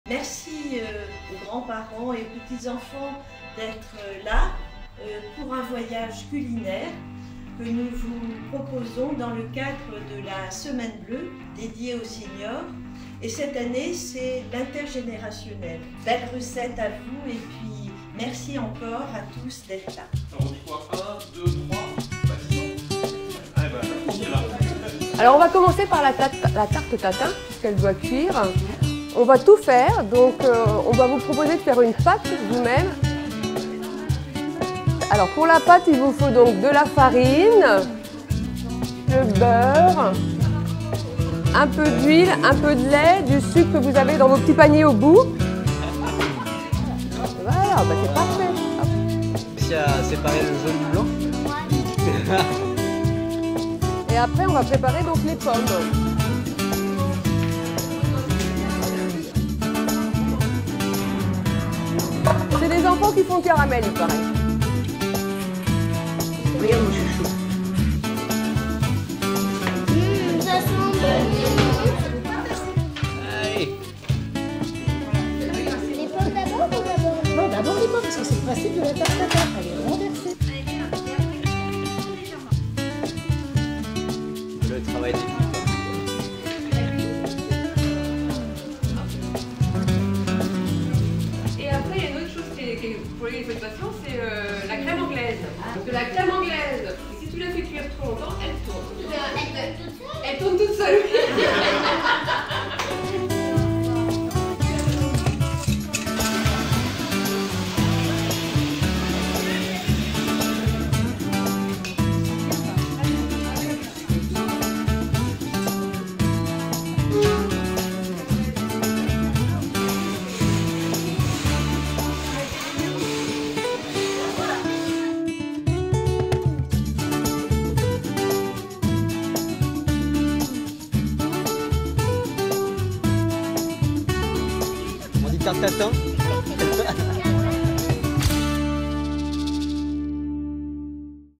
🩵 Semaine Bleue à Montrouge 🍏 La Tarte Tatin
Valoriser la place des aînés et les liens intergénérationnels dans notre société, c'est l'ambition de la Semaine Bleue.

À Montrouge était organisé dans ce sens par la Ville des ateliers cuisine avec des seniors et des jeunes.

Bravo aux équipes d'Ancuisine et à Madame Claude Favra Maire Adjointe à la Famille, à la Parentalité, à la Petite Enfance et au Bien vieillir pour ce beau moment de partage à l'Espace Colucci.